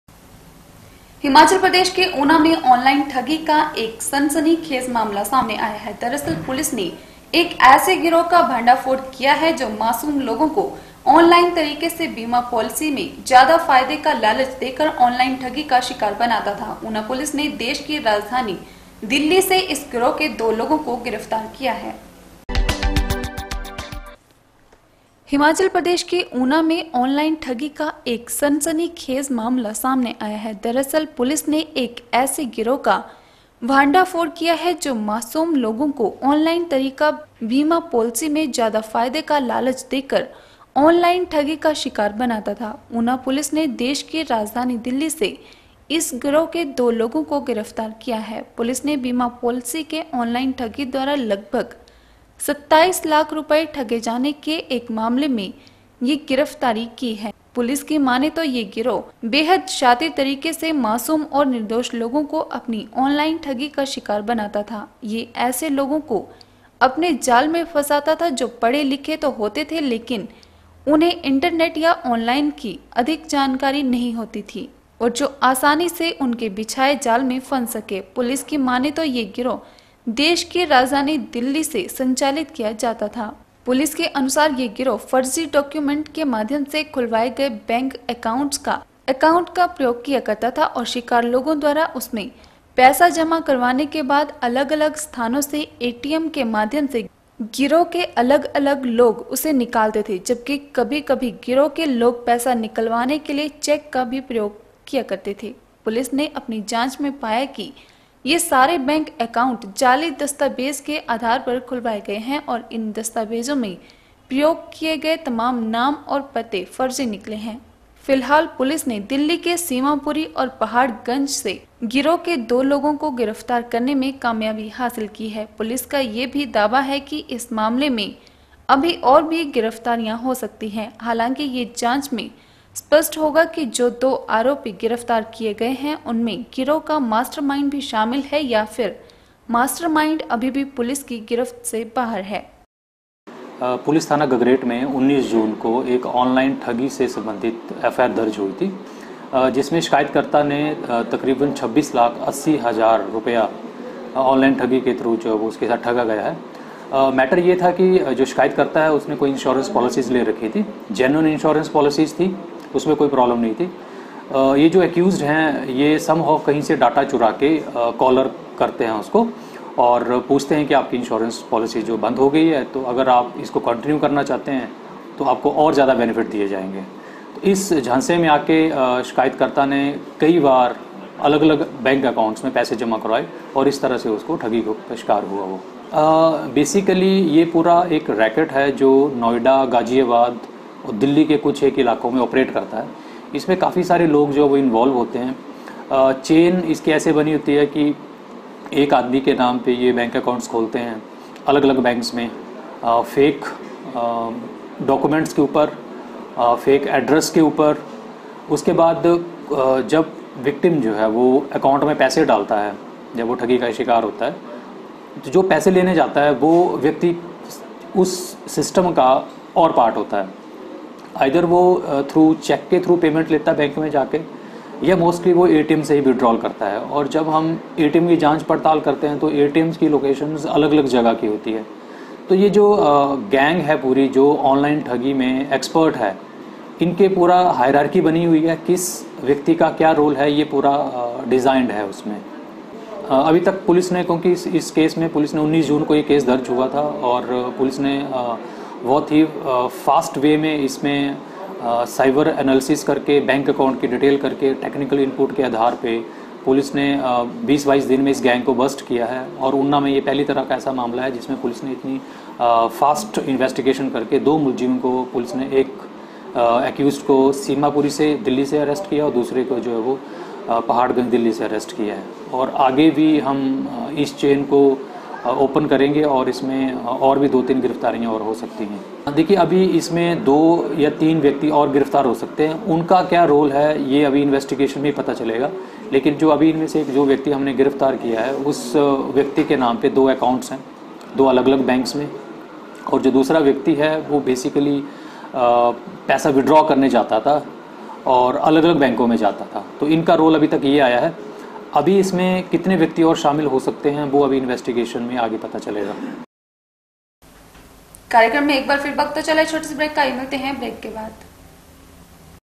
16 JULY N 13 B 3 A sensational case of online shit unfolded in Una
हिमाचल प्रदेश के ऊना में ऑनलाइन ठगी का एक सनसनीखेज मामला सामने आया है । दरअसल पुलिस ने एक ऐसे गिरोह का भंडाफोड़ किया है